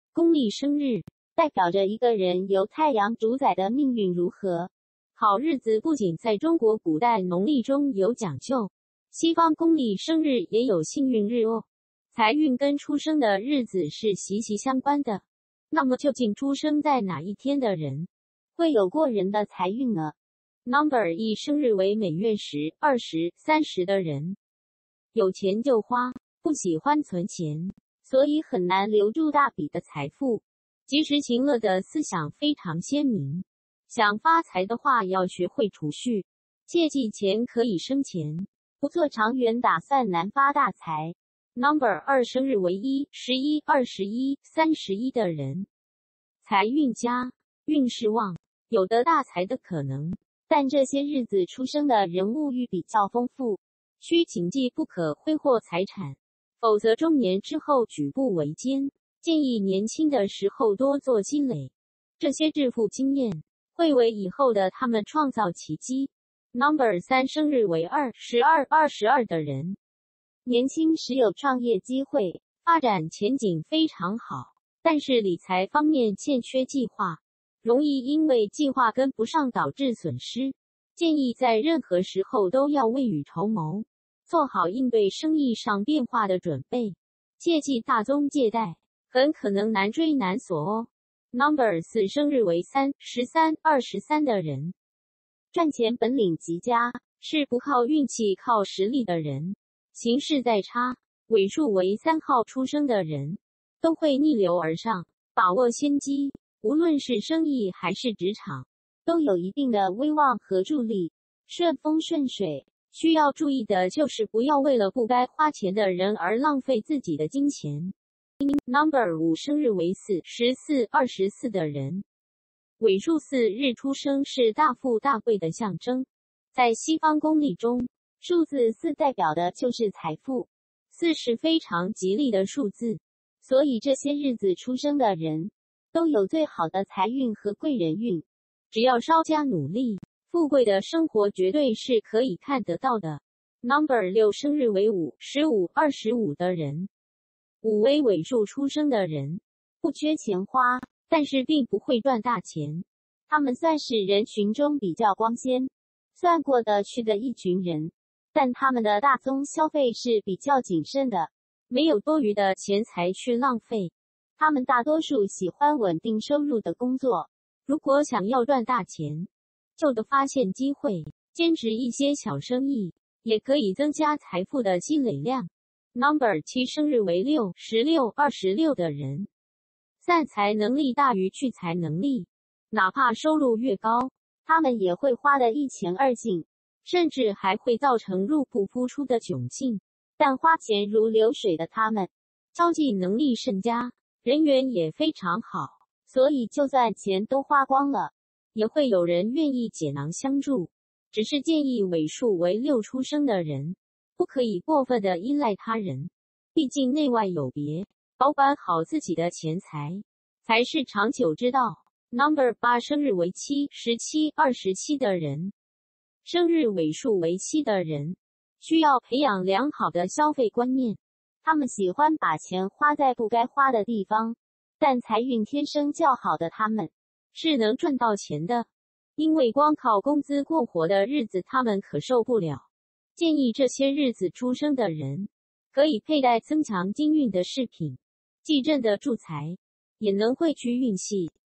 從你的公歷生日就知道你能賺多少錢
公曆生日，代表著一個人由太陽主宰的命運如何，好日子不僅在中國古代農曆中有講究，西方公曆生日也有幸運日哦，財運跟出生的日子是息息相關的，那麼究竟出生在哪一天的人，會有過人的財運呢？
NO·1生日為每月10、20、30的人
有錢就花，不喜歡存錢，所以很難留住大筆的財富，及時行樂的思想非常鮮明，想發財的話要學會儲蓄，切記錢可以生錢，不做長遠打算難發大財。
NO·2生日為1、11、21、31的人
財運佳，運勢旺。有得大財的可能，但這些日子出生的人物慾比較豐富，需謹記不可揮霍財產，否則中年之後舉步維艱。建議年輕的時候多做積累，這些致富經驗，會為以後的他們創造奇蹟。
NO·3生日為2、12、22的人
年輕時有創業機會，發展前景非常好，但是理財方面欠缺計劃，容易因為計劃跟不上導致損失，建議在任何時候都要未雨綢繆，做好應對生意上變化的準備。切忌大宗借貸，很可能難追難索哦。
NO·4生日為3、13、23的人
賺錢本領極佳，是不靠運氣靠實力的人，形勢再差，尾數為3號出生的人，都會逆流而上，把握先機，無論是生意還是職場，都有一定的威望和助力，順風順水，需要注意的就是不要為了不該花錢的人而浪費自己的金錢。
NO·5生日為4、14、24的人
尾數4日出生是大富大貴的象徵，在西方公曆中，數字4代表的就是財富，4是非常吉利的數字，所以這些日子出生的人，都有最好的財運和貴人運，只要稍加努力，富貴的生活絕對是可以看得到的。
NO·6生日為5、15、25的人
5為尾數出生的人,不缺錢花,但是並不會賺大錢,他們算是人群中比較光鮮,算過的去的一群人,但他們的大宗消費是比較謹慎的,沒有多餘的錢財去浪費,他們大多數喜歡穩定收入的工作,如果想要賺大錢,就得發現機會,兼職一些小生意,也可以增加財富的積累量。
NO·7生日為6、16、26的人
散財能力大於聚財能力,哪怕收入越高,他們也會花的一乾二淨,甚至還會造成入不敷出的窘境,但花錢如流水的他們,交際能力甚佳,人緣也非常好,所以就算錢都花光了,也會有人願意解囊相助,只是建議尾數為6出生的人,不可以過分的依賴他人,畢竟內外有別,保管好自己的錢財,才是長久之道。
NO·8 生日為7、17、27的人
生日尾數為7的人,需要培養良好的消費觀念,他們喜歡把錢花在不該花的地方。但財運天生較好的他們,是能賺到錢的,因為光靠工資過活的日子他們可受不了。建議這些日子出生的人,可以佩戴增強金運的飾品,既鎮得住財,也能匯聚運氣。
NO·9 生日為8、18、28的人
心眼非常大,野心更是無限大的日子尾數8生人,一輩子絕對不可能安於平淡的生活,他們的吸金能力出眾,可發大財,但也可能走錯了歪路導致一貧如洗。如果想要發財,切記一個字「穩」,不要讓過分膨脹的野心沖昏了頭腦,穩穩噹噹方可長久,發財不能急躁,提防小人讒言,是尾數為8出生人最需要注意的。
NO·10生日為9、19、29的人
9為尾數出生的人多數是喊著金湯匙長大的,他們多數都會生活在富裕的家庭,或年輕時就有一筆現成的財富,不需要他們自己過多的奮鬥,但是,尾數為9出生的人,容易帶有一種驕傲自滿的感覺,這對他們日後的發展不利,如果繼續保持驕傲自滿,而不思進取或不尊重他人,也是很容易坐吃山空的,把握自己的命運,讓錢生錢,才是正途。